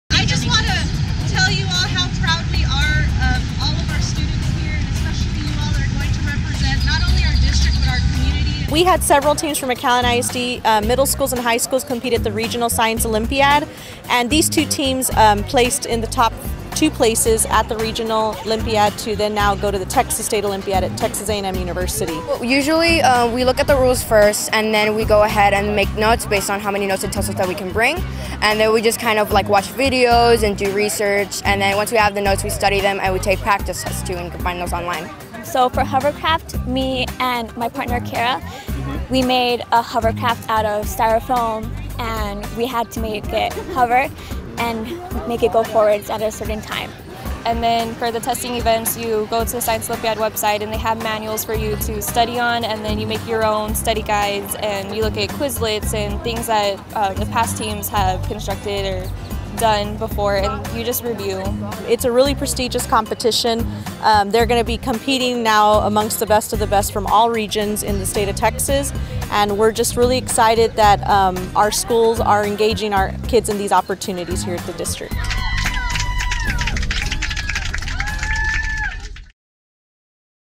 Texas Science Olympiad | McAllen ISD
Teams from Lamar Academy and Fossum Middle School qualified to compete with the best of the best at the Texas Science Olympiad April 20-21, 2018, in College Station. Several students are competing in multiple categories that include biology, chemistry, physics, engineering, and astronomy. Any students interested in joining a science team can reach out to one of their science teachers at their campus.